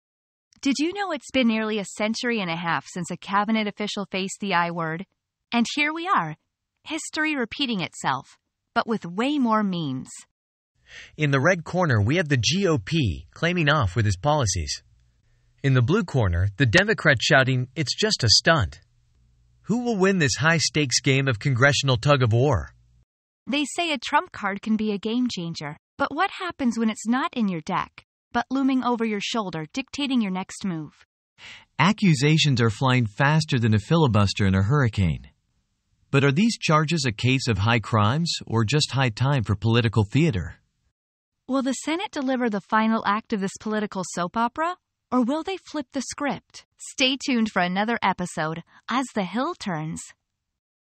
Can You Spell 'Impeachment'? House Panel Moves Against Mayorkas!
Gear up for a political rollercoaster as we dive into the uproarious world of the House panel's impeachment move against Alejandro Mayorkas! Will it stick, or will it tick? Tune in for a chuckle-rich journey through the halls of Congress.